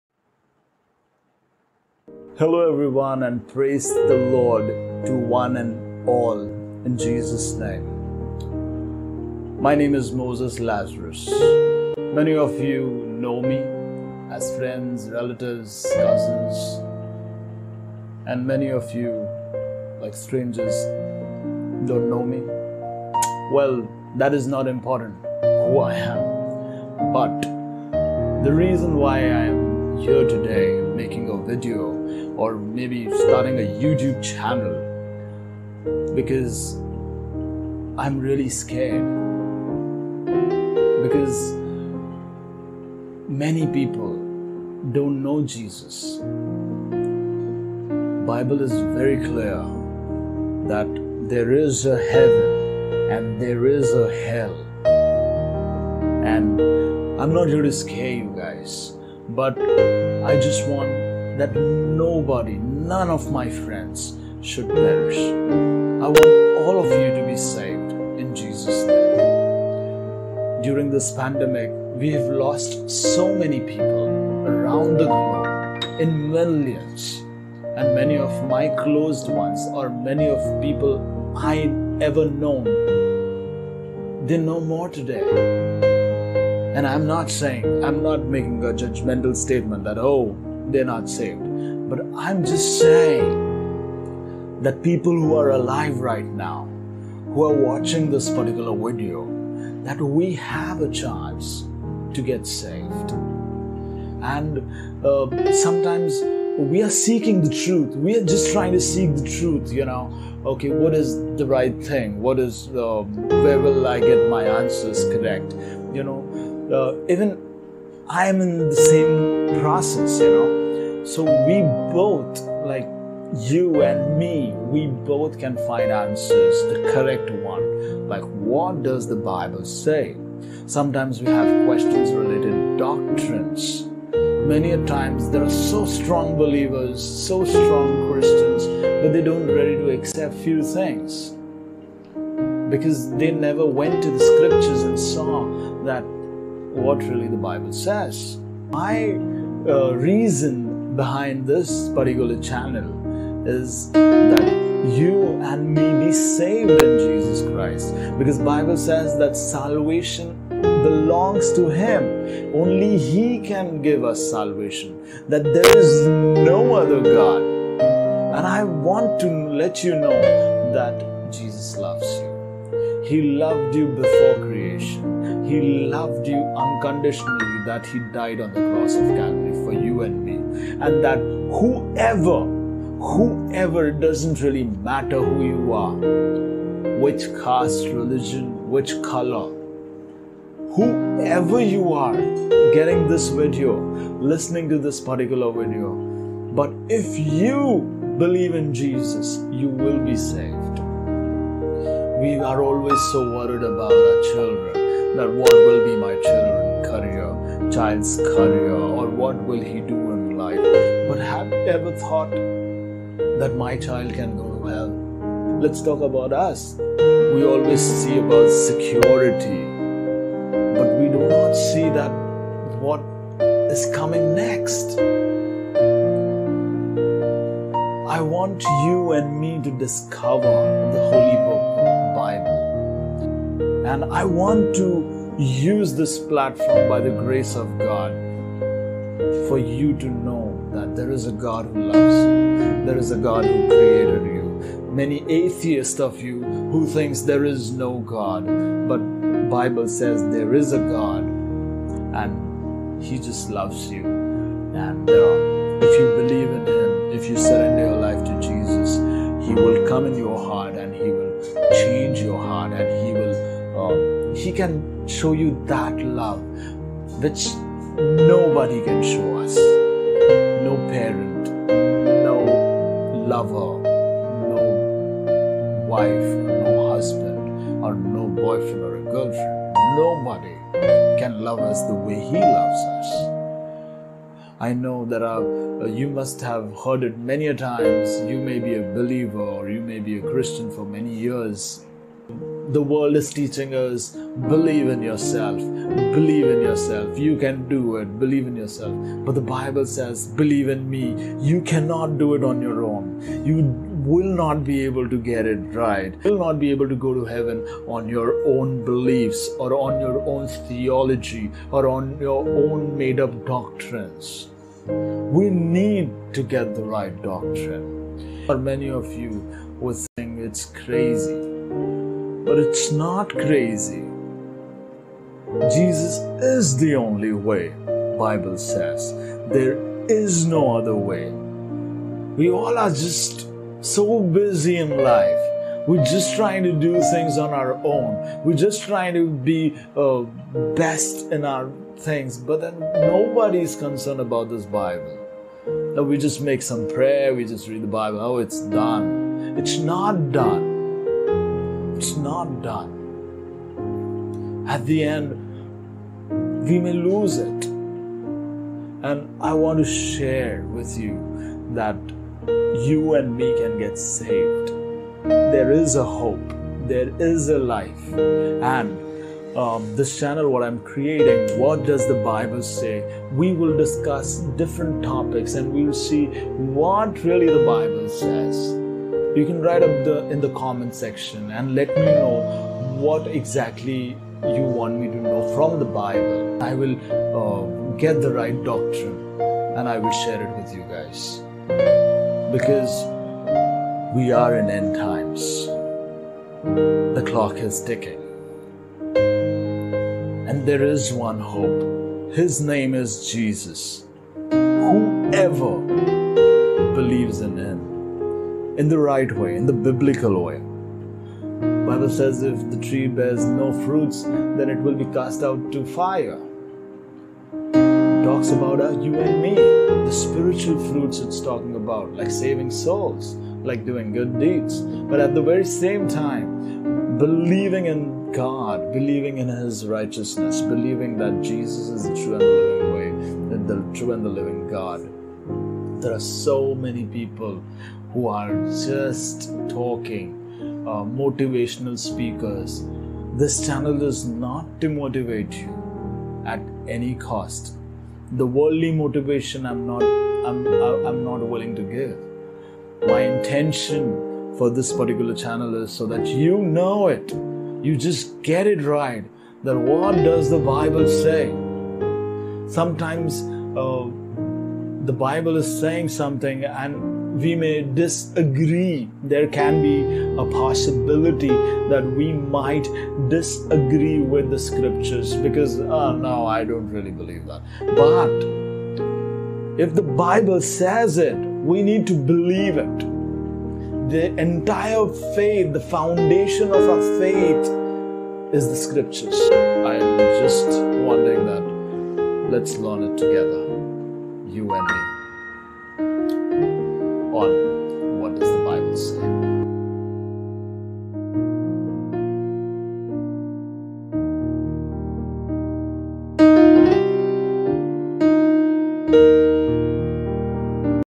Intro Video - WHAT DOES THE BIBLE SAY?
This Video is to share the love of Christ and let people know that there is Heaven and there is Hell
Get the correct Biblical doctrine only on "WHAT DOES THE BIBLE SAY?"